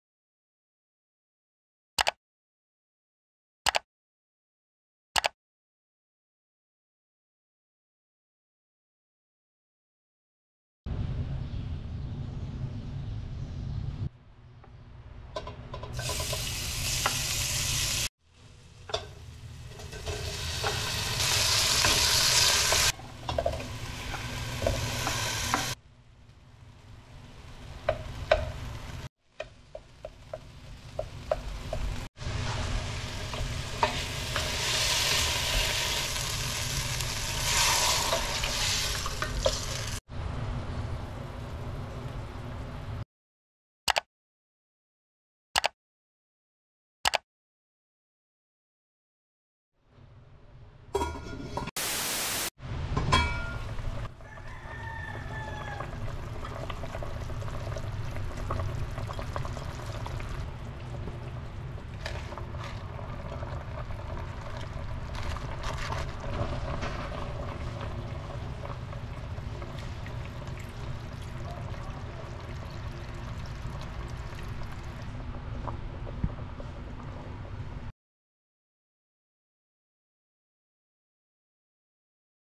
Craving for SINIGANG? | ASMR Cooking | Aling Lyne’s Kitchen
Step by steo recipe of how to cook sinigang na pampano sa miso.